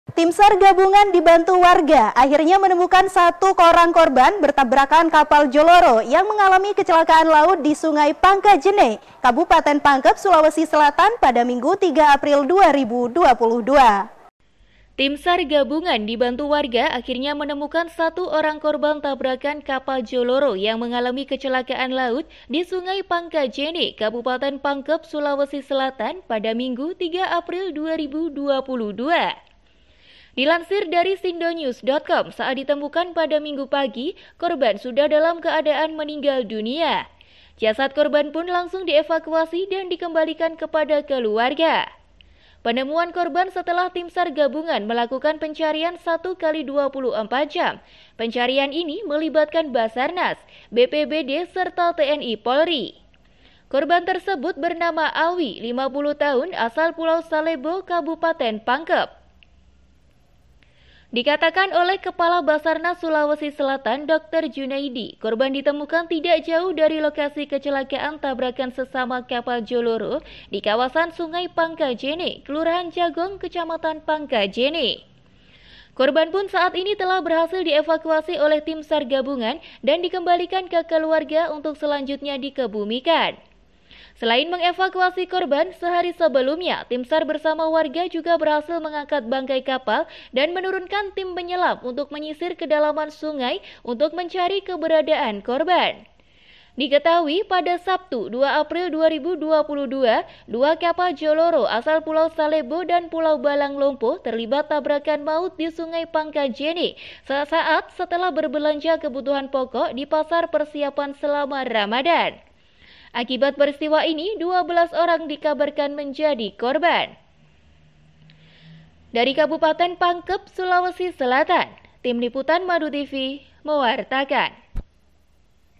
Korban Kecelakaan Laut yang Melibatkan 2 Unit Kapal Berhasil Dievakuasi
Pangkep - Tim sar gabungan dibantu warga akhirnya menemukan satu orang korban tabrakan Kapal Jolloro yang mengalami kecelakaan laut di Sungai Pangkajenne, Kabupaten Pangkep, Sulawesi Selatan, Minggu (3/4/2022).

Dilansir dari sindonews.com, saat ditemukan pada Minggu pagi, korban sudah dalam keadaan meninggal dunia. Jasad korban pun langsung dievakuasi dan dikembalikan kepada keluarganya. Penemuan korban setelah tim sar gabungan melakukan pencarian 1x24 jam yang melibatkan basarnas, BPBD serta TNI-Polri.

Korban diketahui bernama Alwi (50) asal Pulau Salebbo, Kabupaten Pangkep. Selain mengevakuasi korban, sehari sebelumnya tim sar bersama warga juga berhasil mengangkat bangkai kapal dan menurunkan tim penyelam untuk menyisir kedalaman sungai untuk mencari keberadaan korban.

Diketahui pada Sabtu (2/4/2022), dua Kapal Jolloro asal Pulau Salebbo dan Pulau Balang Lompo terlibat tabrakan maut di Sungai Pangkajenne. Akibat perisitwa tersebut dinyatakan sebanyak 12 orang menjadi korban. (red)